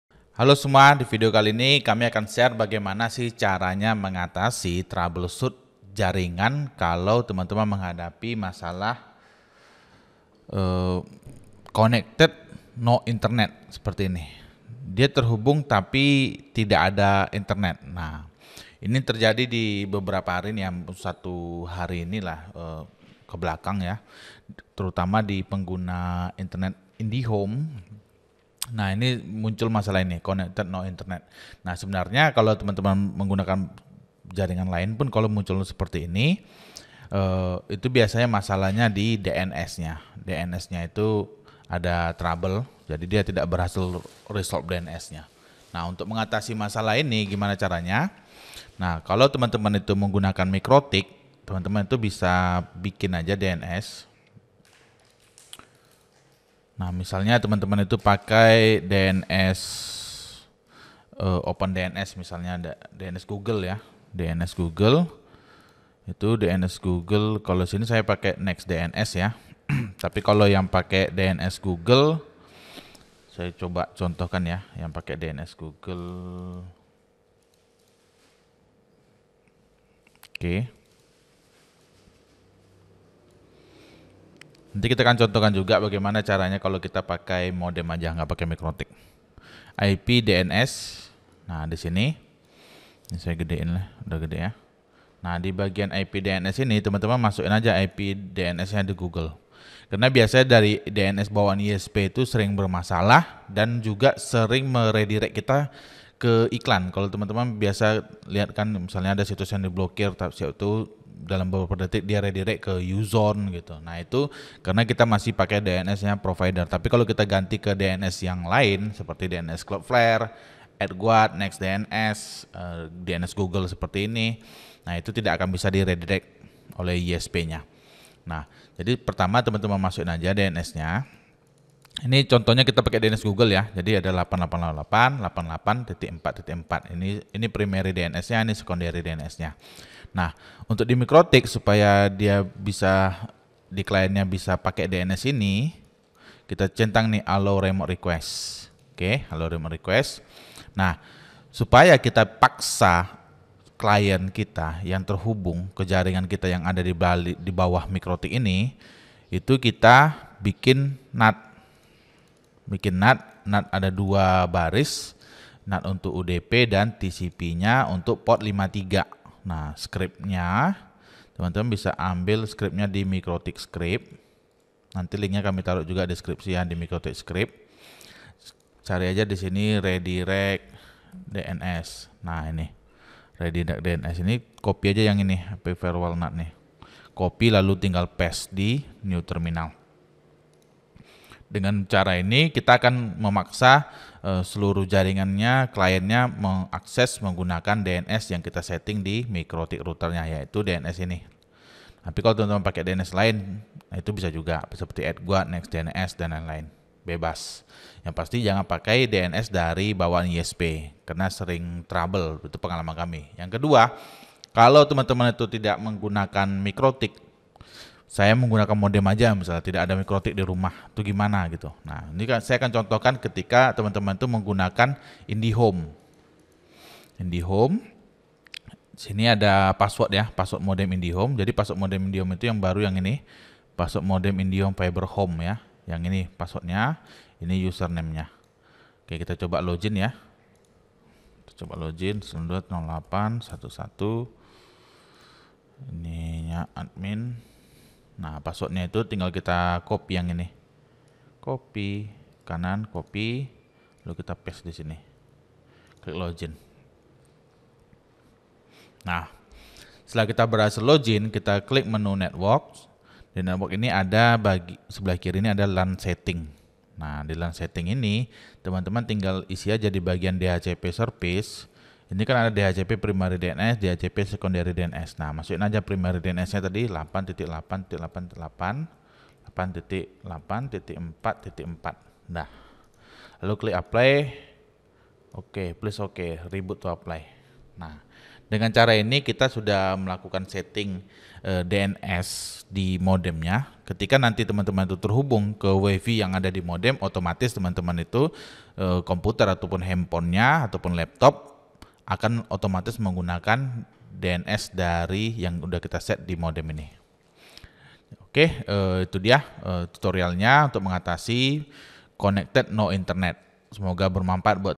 Cara Mengatasi Masalah WIFI No Internet Access Dijamin 100% Berhasil
🔥Eksklusif untuk Anda! Tutorial Premium yang hanya kami share via E-mail
# di video ini kami membagikan Cara Mengatasi Masalah WIFI No Internet Access Dijamin 100% Berhasil